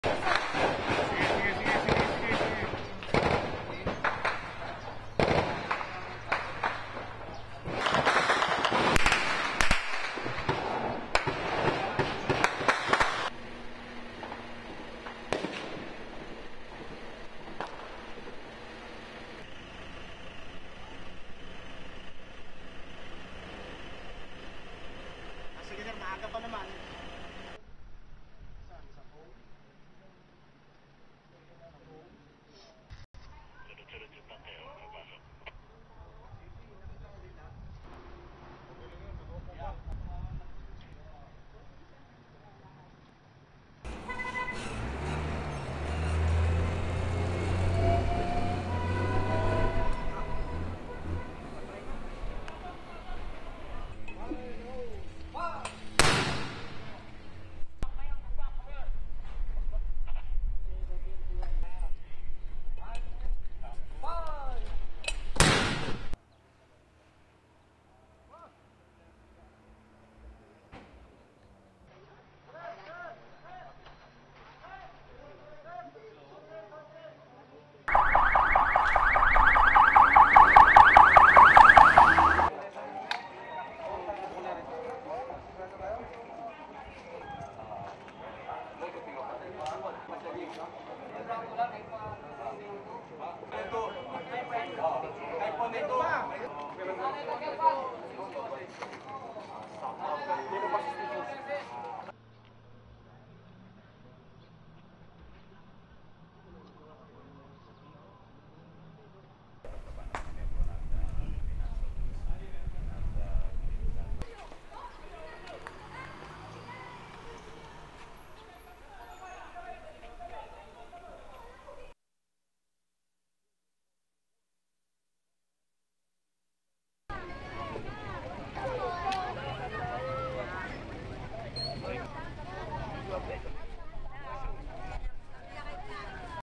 WATCH: Zamboanga Siege, Day 8
Government forces conducted a major offensive against the group of Moro National Liberation Front (MNLF) founding chairman Nur Misuari on Monday.